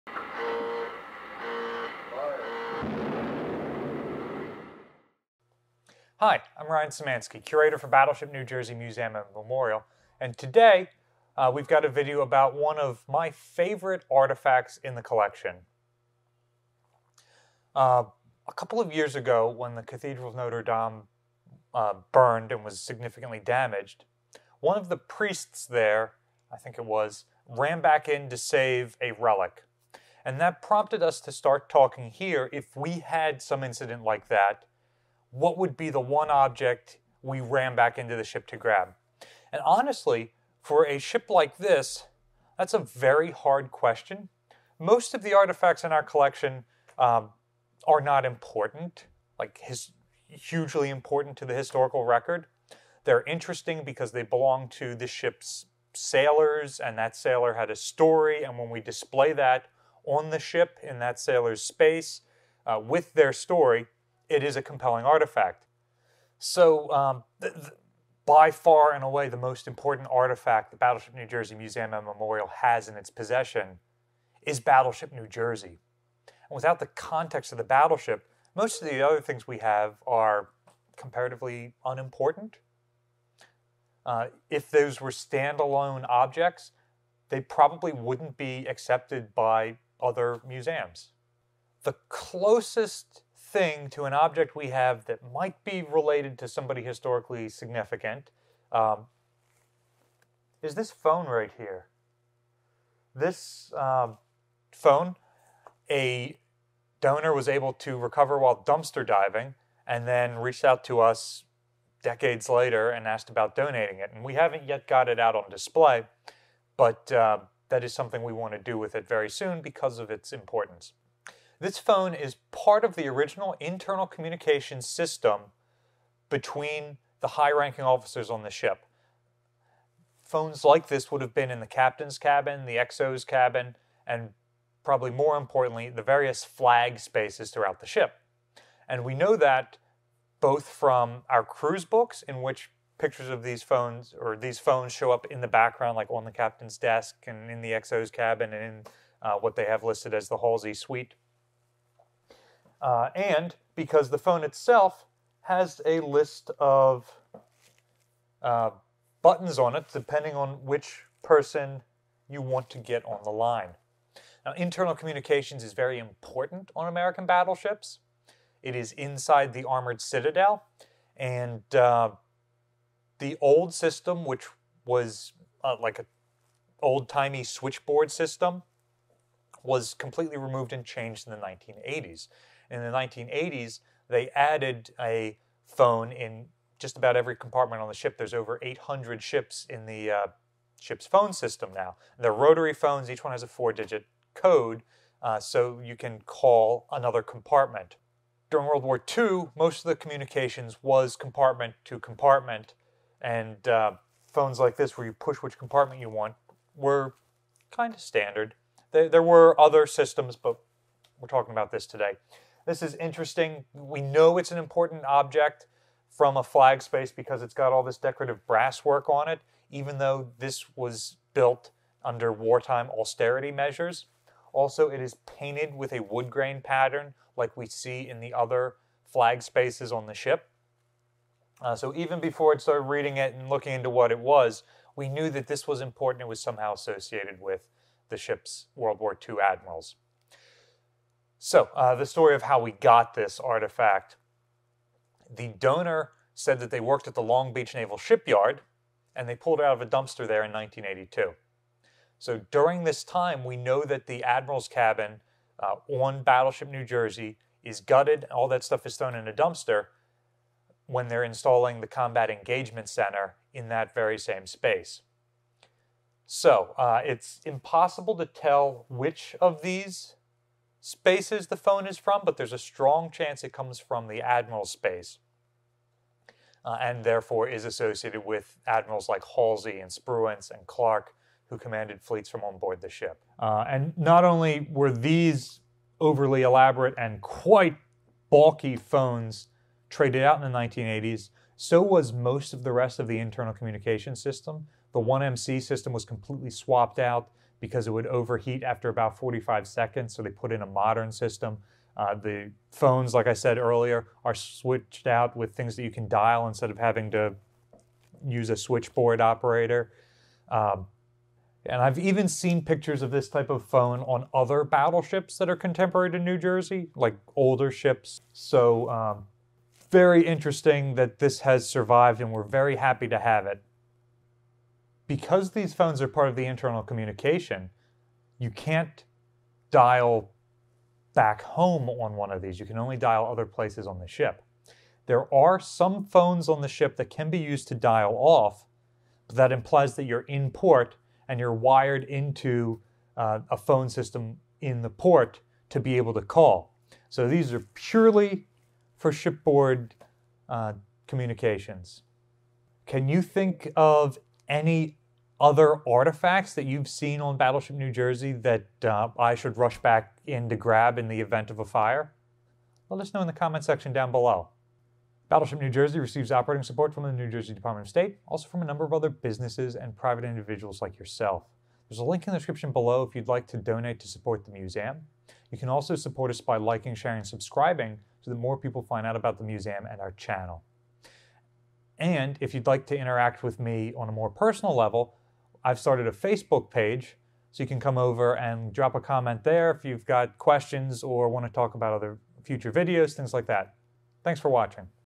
The Admiral's Phone
In this episode we're down in the collection's space talking about a special phone.